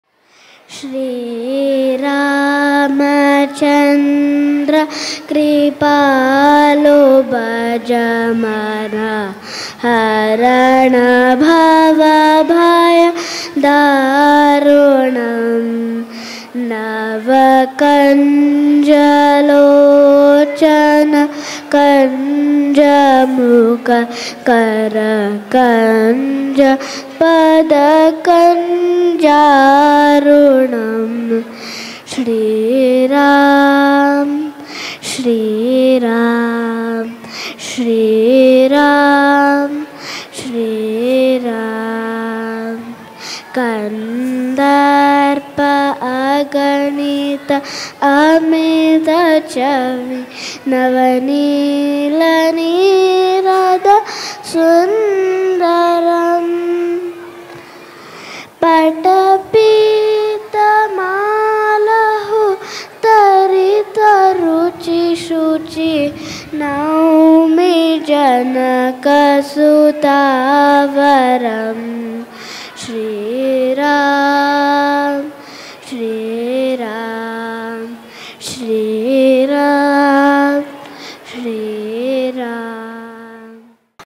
Tenbroeck Academy's Musical Magic on World Music Day! | Inchara's Vocal Performance
Inchara, a young prodigy from Tenbroeck Academy, mesmerizes with her soulful voice. Celebrate International Music Day with this enchanting vocal performance. Witness the magic of music unfold as Inchara's talent shines bright.